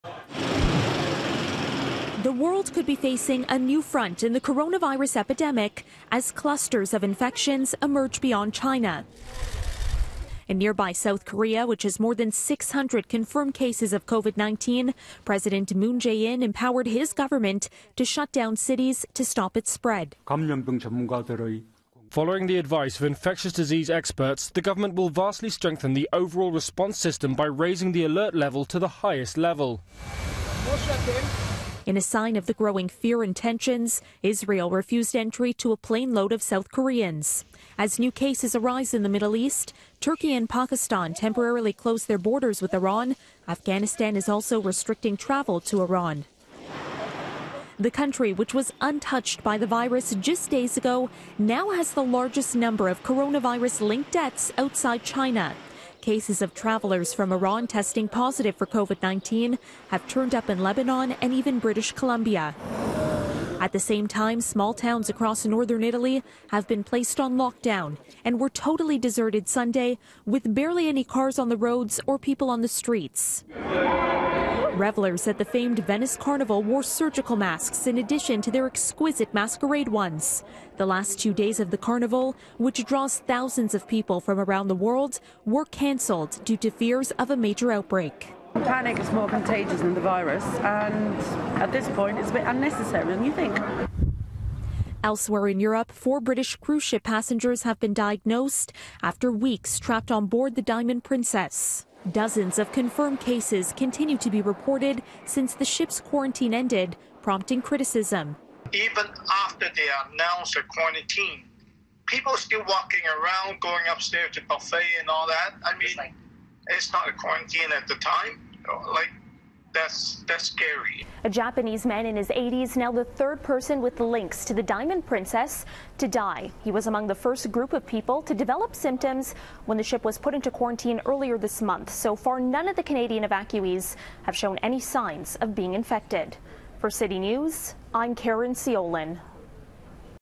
Countries around the world battling coronavirus
As a coronavirus outbreak is growing beyond China, so is fear. Caryn Ceolin with the dramatic efforts countries around the world are taking to stop its spread.